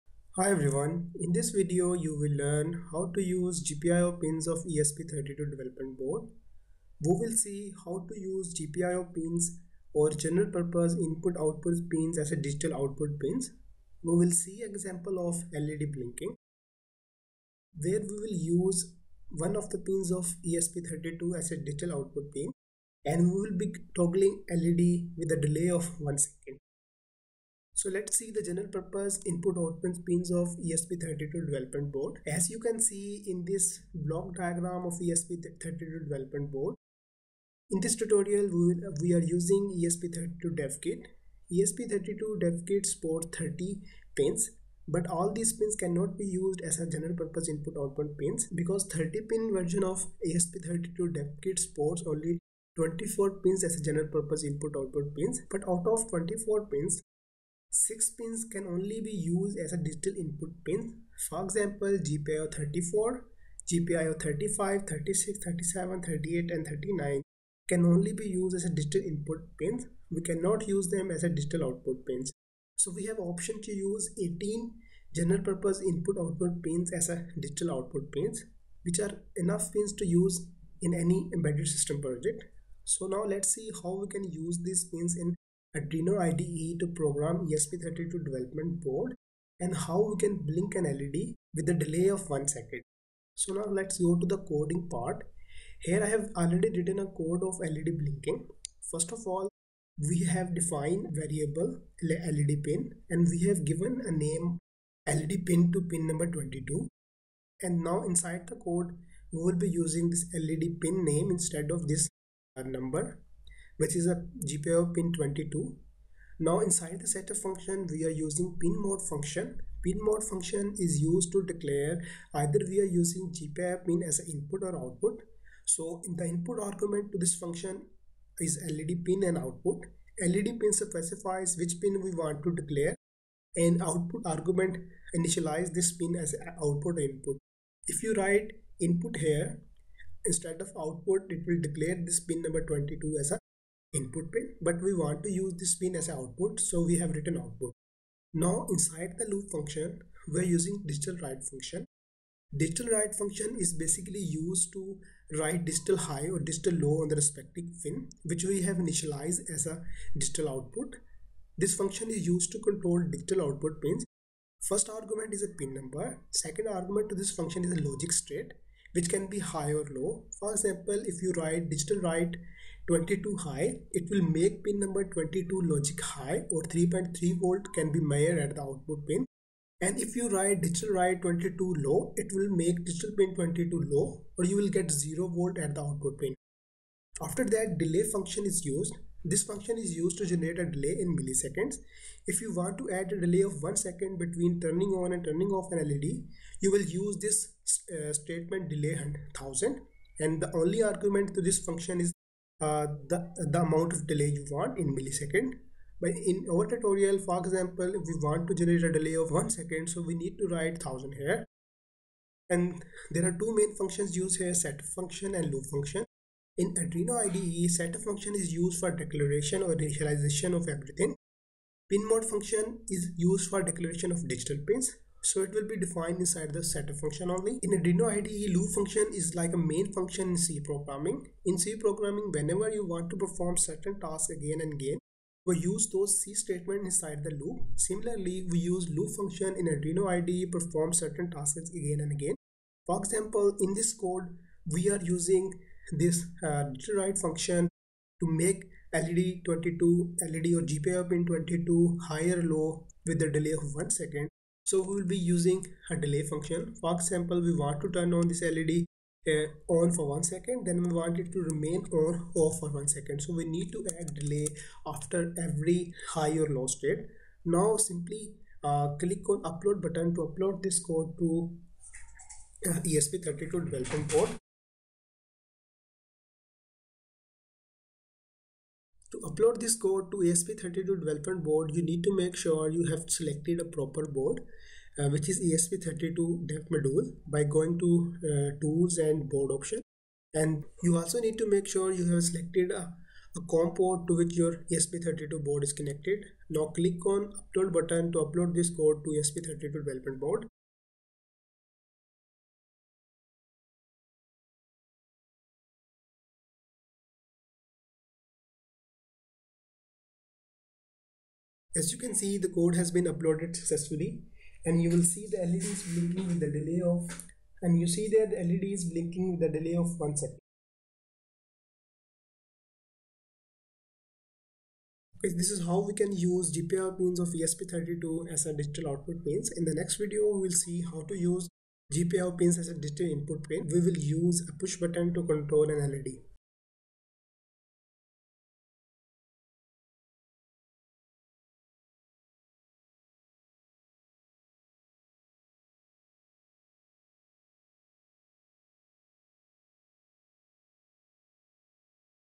LED blinking ESP32: How to use GPIO pins as a digital output pins
How to use GPIO pins of ESP32 development board as digital output pins,

How to use GPIO pins of ESP32 devkit with LED blinking example. Whenever any beginner starts learning about any microcontroller based development board, Experts always recommends beginners to start with LED blinking.